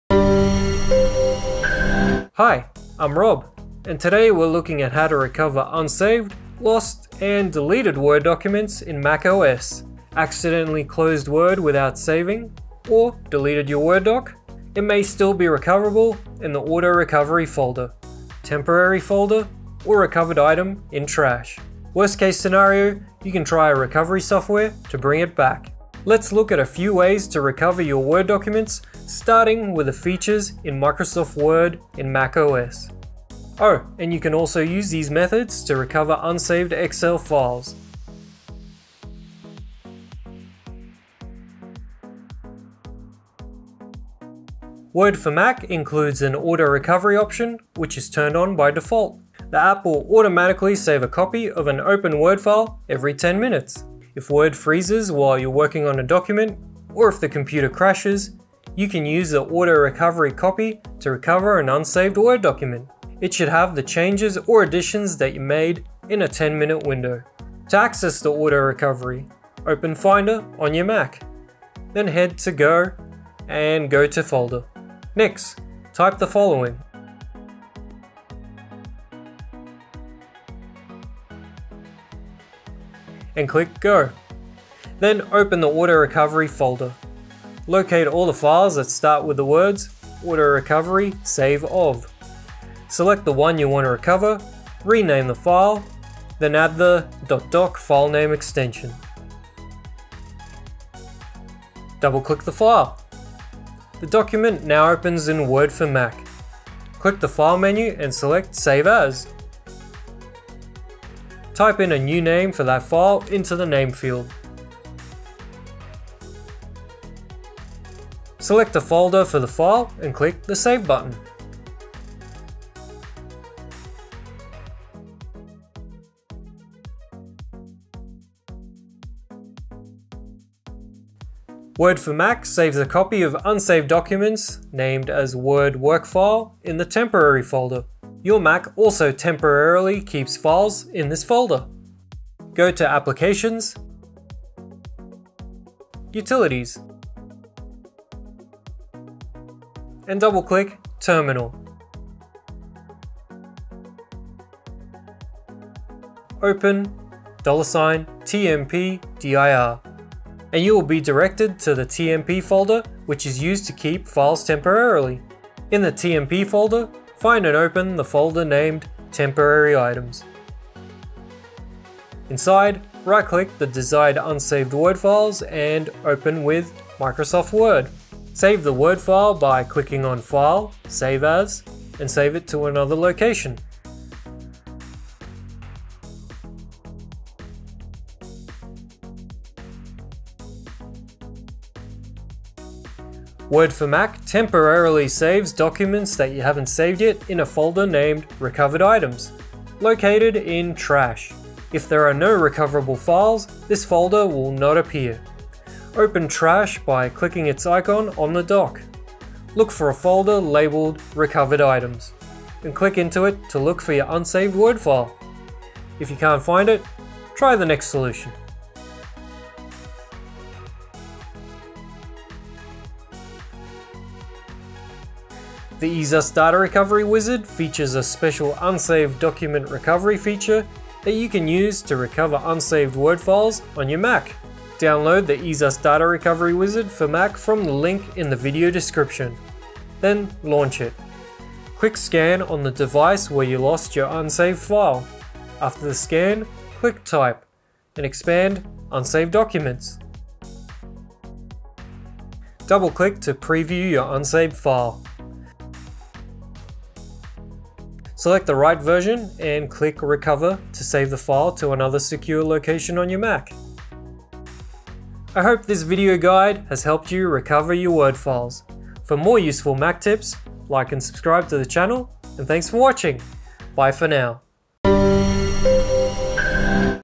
Recover unsaved Word Documents on Mac!
In this video we take a look at how you can recover unsaved Word files on Mac, whether it's using Word AutoRecovery, TMP directory, Trash or recovery software.

00:00 - Options for Word Recovery
00:37 - AutoRecovery in Word
02:04 - Recover Doc From TMP Folder
03:06 - Recover Doc From Mac Trash
03:39 - Recovery Software